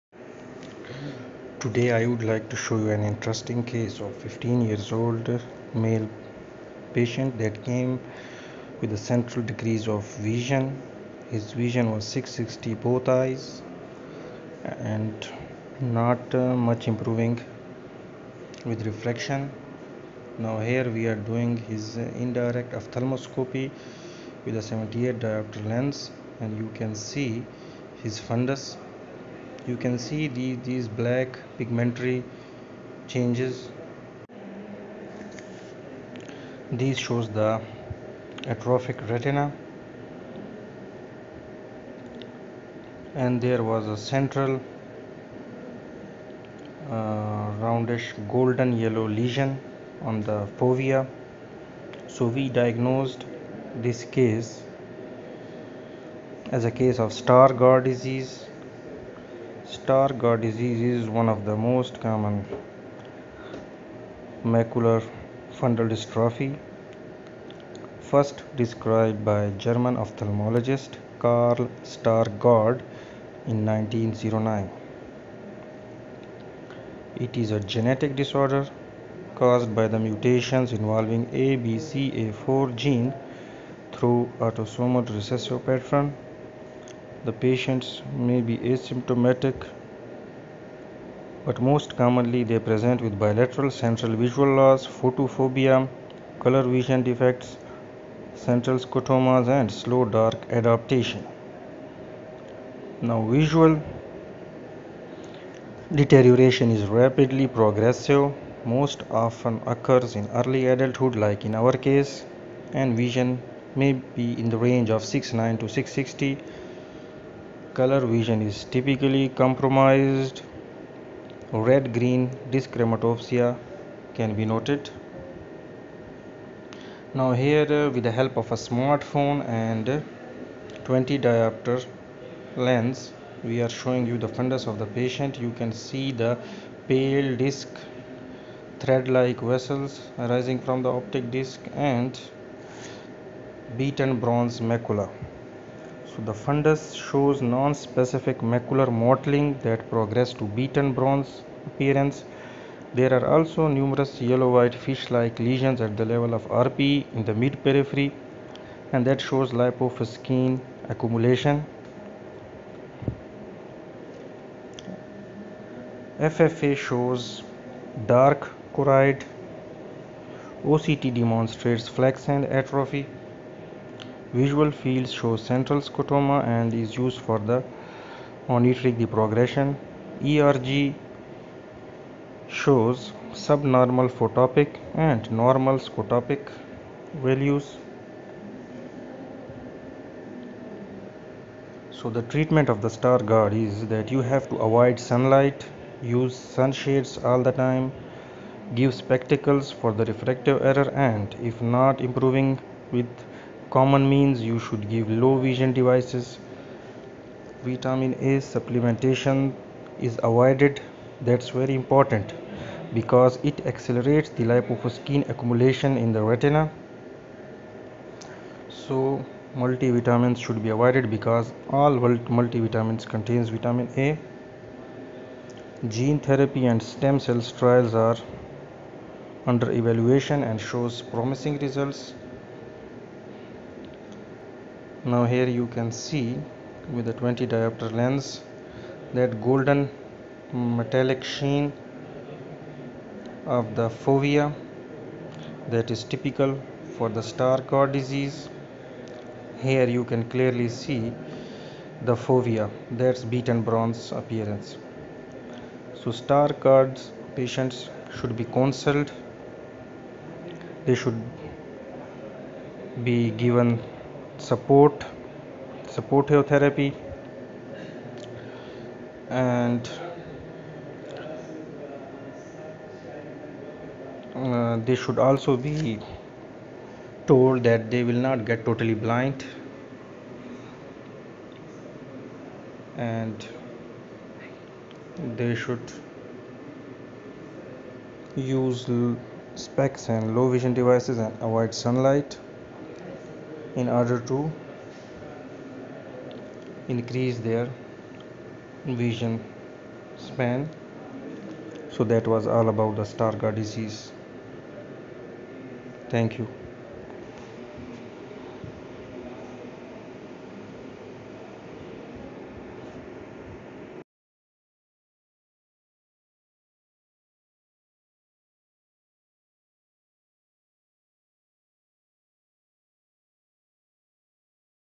Stargardt disease
Today we described a case of stargardt disease in detail